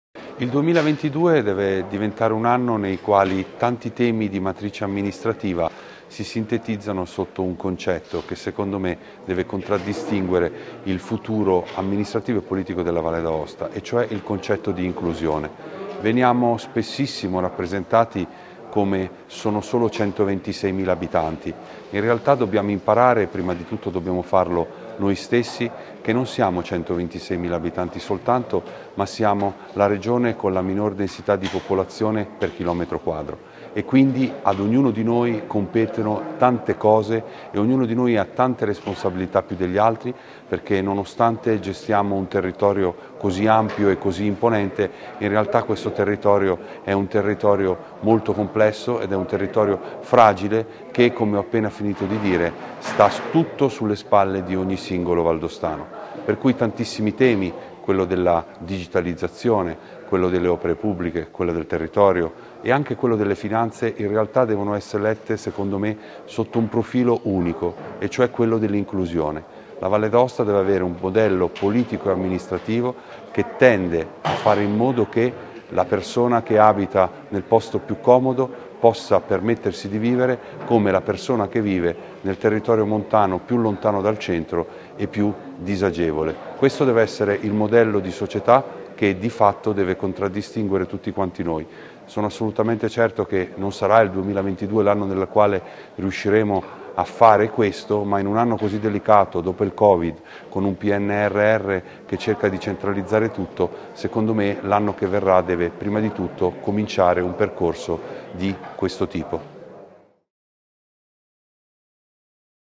Conferenza stampa di fine anno - Carlo Marzi
Conferenza stampa di fine anno - Carlo Marzi, Assessore alle Finanze, Innovazione, Opere pubbliche e Territorio